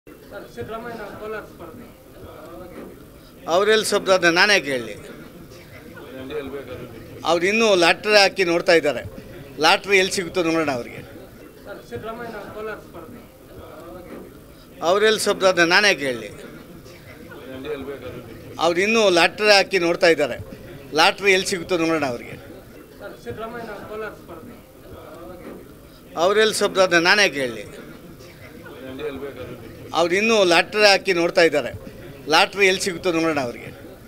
ಸಿದ್ದರಾಮಯ್ಯನವರು ಲಾಟರಿ ಹಾಕಿ ನೋಡುತಿದ್ದಾರೆ ಲಾಟರಿ ಎಲ್ಲಿ ಸಿಗುತ್ತೋ ನೋಡೋಣಾ - ಕೇಂದ್ರ ಸಚಿವ ಎ.ನಾರಾಯಣಸ್ವಾಮಿ
A Narayanaswamy : ಸಿದ್ದರಾಮಯ್ಯನವರು ಲಾಟರಿ ಹಾಕಿ ನೋಡುತಿದ್ದಾರೆ ಲಾಟರಿ ಎಲ್ಲಿ ಸಿಗುತ್ತೋ ನೋಡೋಣಾ - ಕೇಂದ್ರ ಸಚಿವ ಎ.ನಾರಾಯಣಸ್ವಾಮಿ

ಕೋಲಾರದಲ್ಲಿ ಮಾಜಿ ಸಿಎಂ ಸಿದ್ದರಾಮಯ್ಯ ಸ್ಪರ್ಧೆ ವಿಚಾರವಾಗಿ ಕೇಂದ್ರ ಸಚಿವರಾದ ಎ.ನಾರಯಣಸ್ವಾಮಿರವರು ಅನೇಕಲ್‌ನಲ್ಲಿ ಪ್ರತಿಕ್ರಿಯೆ ನೀಡಿದ್ದಾರೆ. ಸಿದ್ದರಾಮಯ್ಯನವರು ಲಾಟರಿ ಹಾಕಿ ನೋಡುತಿದ್ದಾರೆ ಲಾಟರಿ ಎಲ್ಲಿ ಸಿಗುತ್ತೋ ನೋಡೋಣಾ ಎಂದು ಕೇಂದ್ರ ಸಚಿವ ಎ.ನಾರಾಯಣಸ್ವಾಮಿರವರು ಹೇಳಿಕೆ ನೀಡಿದ್ದಾರೆ.